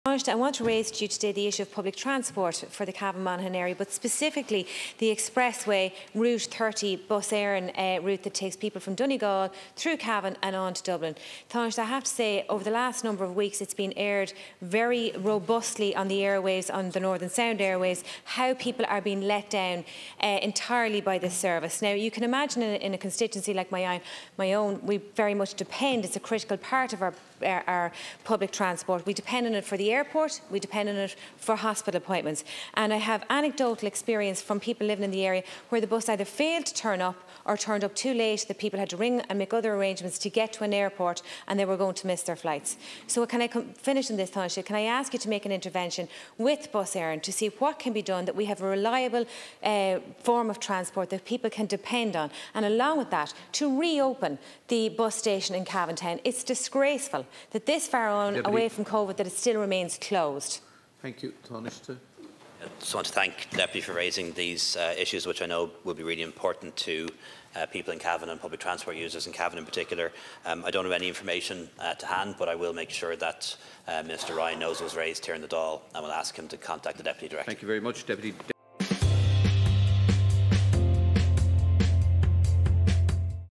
Expressway bus service disruption raised in Dáil
We need reliable public transport here in Cavan-Monaghan.

Last Thursday I called on the Tánaiste to examine the Expressway route from Donegal through Cavan to Dublin after the latest spate of no-shows and delays.
I also called for the bus station in Cavan town to reopen.
Watch 👇